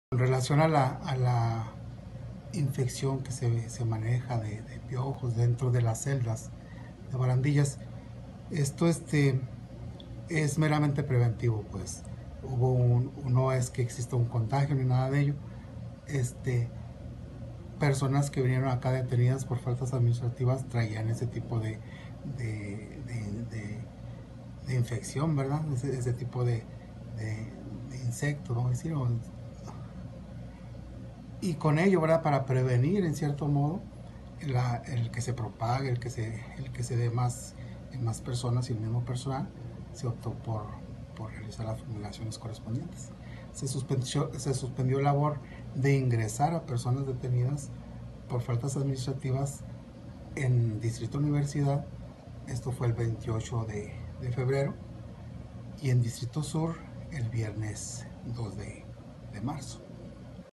Infestan piojos estación de policía municipal
Ernesto Frías Galván -encargado de oficialía jurídica y Barandilla- declaró que efectivamente algunos de los detenidos que cayeron por faltas administrativas “traían ese tipo de infección”.